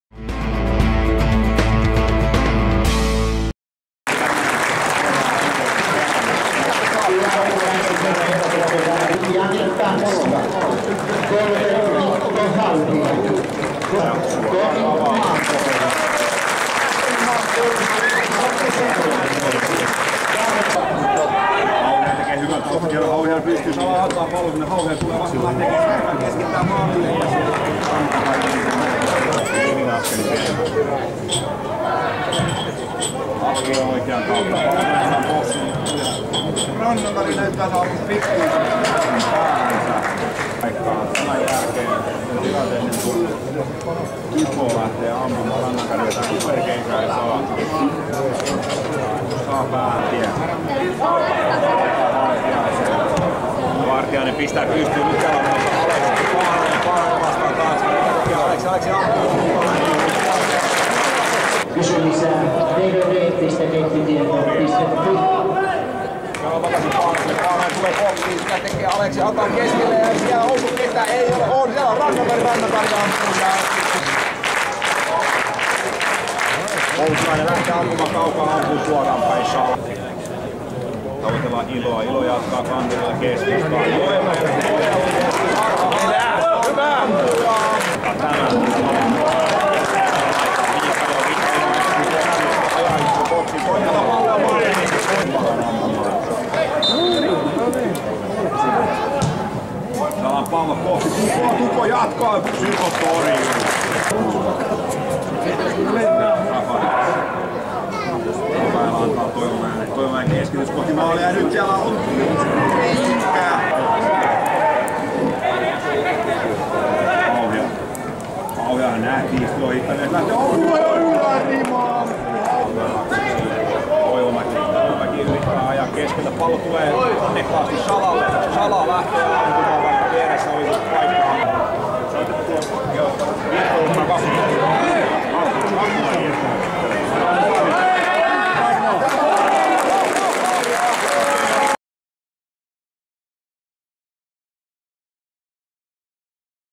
Liigacup: KuPS - FC Lahti kooste
Kooste ottelusta KuPS - FC Lahti. Ottelu päättyi 0-0 ja oli samalla Tomi Maanojan ensiesiintyminen kotiyleisön edessä.